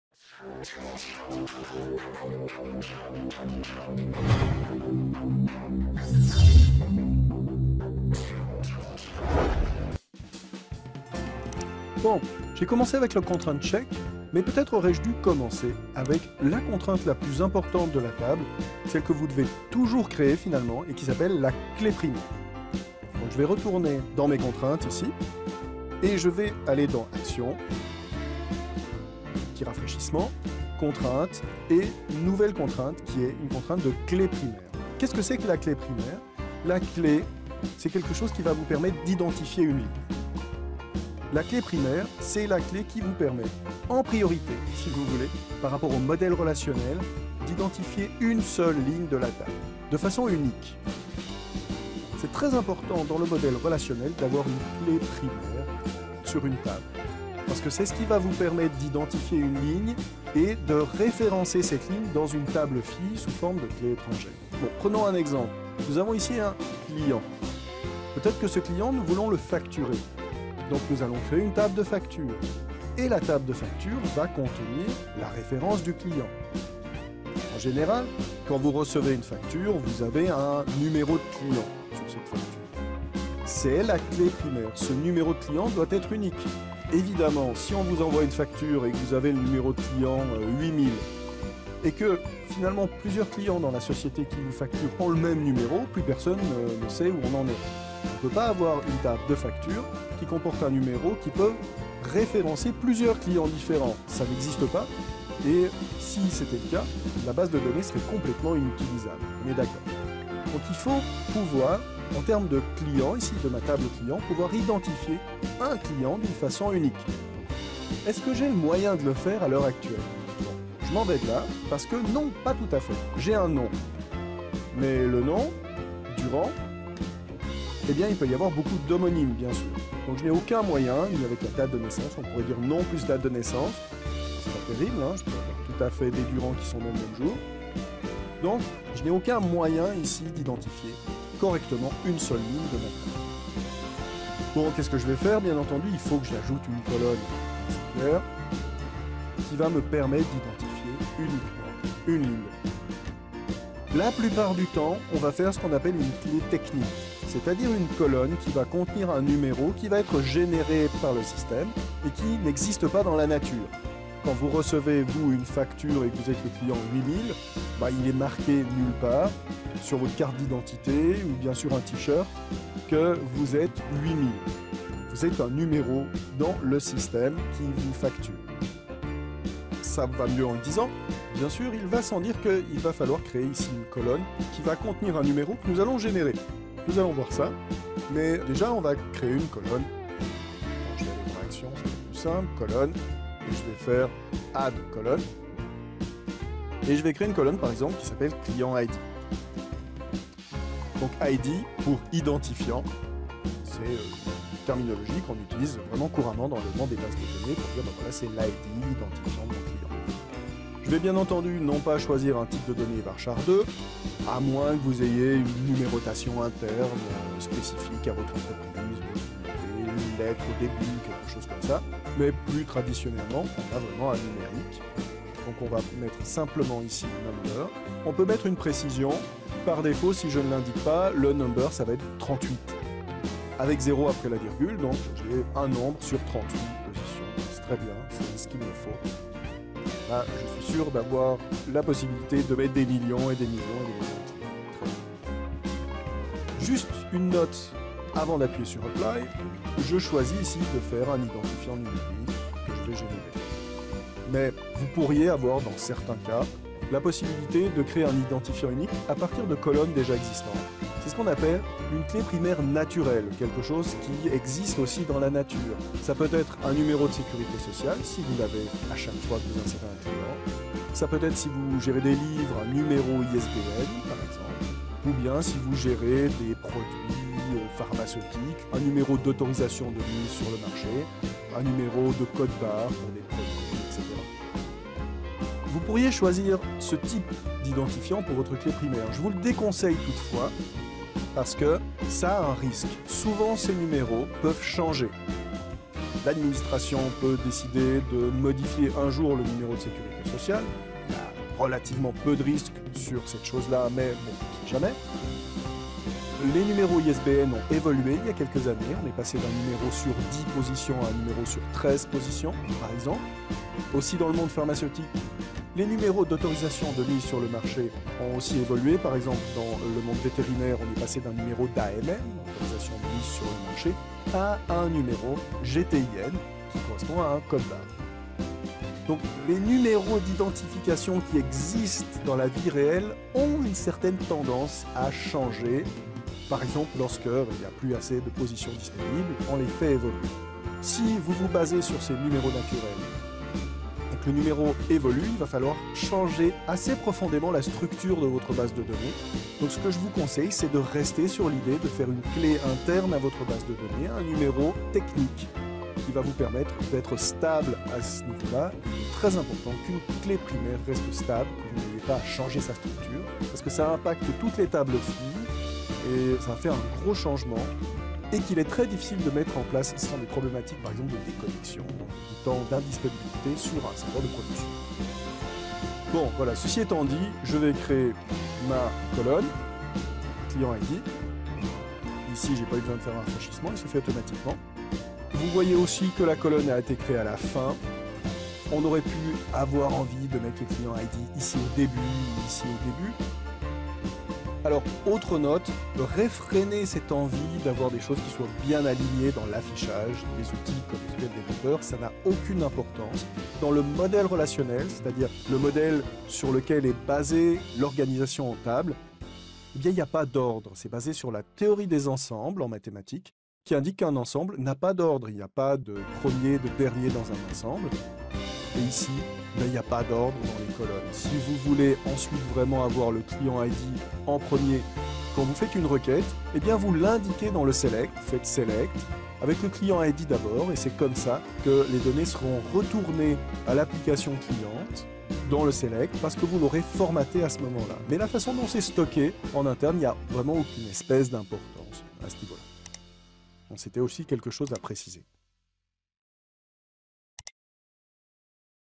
Lesson 12 : tutorial Se servir de colonnes d'identifiant Oracle Database 12
Explore the most powerful and used relational database engine! With Rudi Bruchez, you'll learn how to create tables and constraints, but also views and triggers among other code objects. You will also learn how to write queries adapted to the Oracle SQL dialect. This will allow you to easily manage your databases in Oracle Database 12.